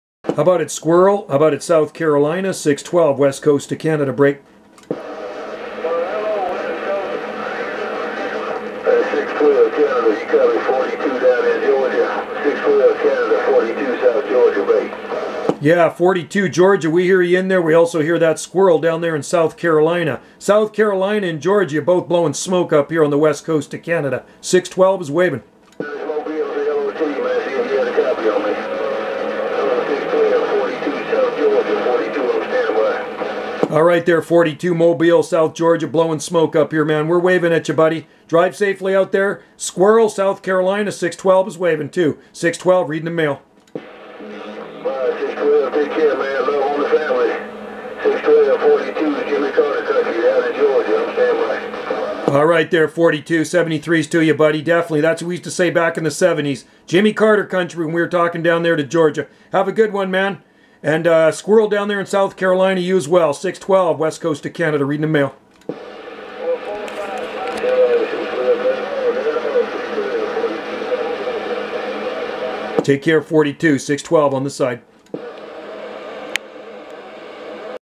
CB radio skip shooting Squirrel in South Carolina & 42 Georgia waving!
Shooting am skip on channel 35